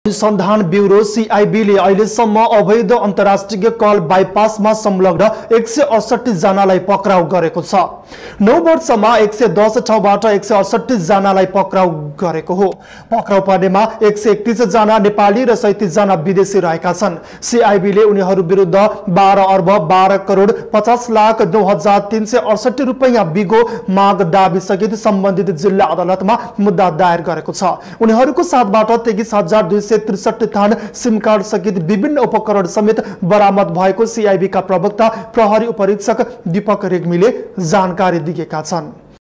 कल बाइपासमा संलग्न १ सय ६८ जनालाई पक्राउ
केन्द्रीय अनुसन्धान ब्यूरो सीआइबीले अहिलेसम्म अवैध अन्तर्रा्ष्ट्रिय कल बाइपासमा संलग्न १ सय ६८ जनालाई पक्राउ गरेको छ ।

९ बर्षमा १ सय १० ठाउँबाट १ सय ६८ जना पक्राउ परेका हुन् । पक्राउ पर्नेमा १ सय ३१ जना नेपाली र ३७ जना विदेशी रहेका छन् । सीआइबीले उनीहरुविरुद्ध १२ अर्ब, १२ करोड, ५० लाख ९ हजार ३ सय ६८ रुपैयाँ बिगो मागदावी सहित सम्बन्धित जिल्ला अदालतमा मुद्धा दायर गरेको छ । उनीहरुको साथबाट २३ हजार २ सय ६३ थान सिमकार्ड सहित विभिन्न उपकरण समेत बरामद भएका सीआइबीका प्रवक्ता प्रहरी उपरीक्षक दिपक रेग्मीले जानकारी दिनुभयो ।।